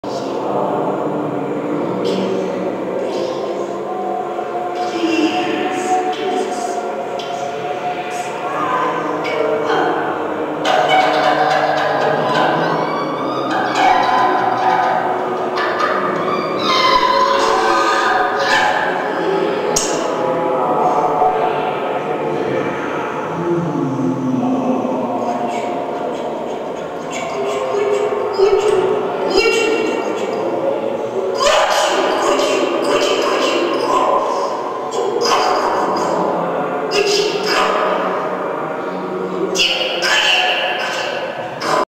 Tony Oursler, at Magasin 3
"Caricature", 2002.
Plastic, paint, video projector, DVD-player, performance by Constance DeJong. Exhibited at Magasin 3 Stockholm Konsthall Sep-Dec, 2002